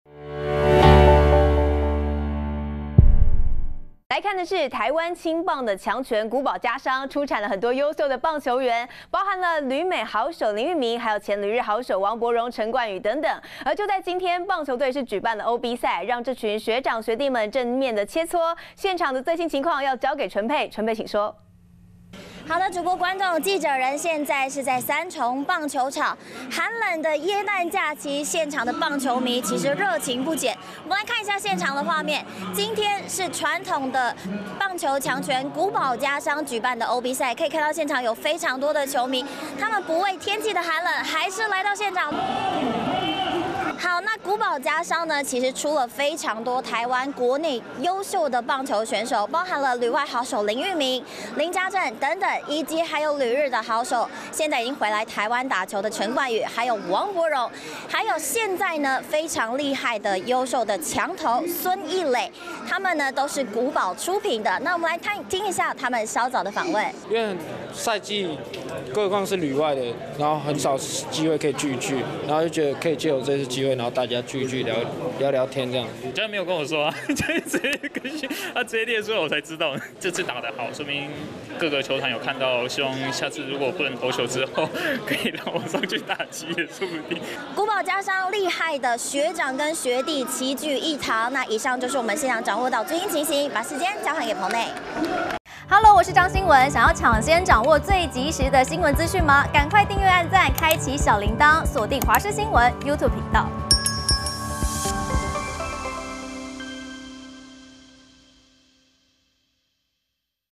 穀保OB賽陣容豪華 王柏融.林昱珉都出席｜華視新聞 20231224
台灣青棒的強權穀保家商出產很多優秀棒球員，包含了旅美好手林昱珉、前旅日好手王柏融、陳冠宇等等。而就在今天，棒球隊舉辦了OB賽，讓這些學長學弟們正面切磋，現場情形交給淳霈。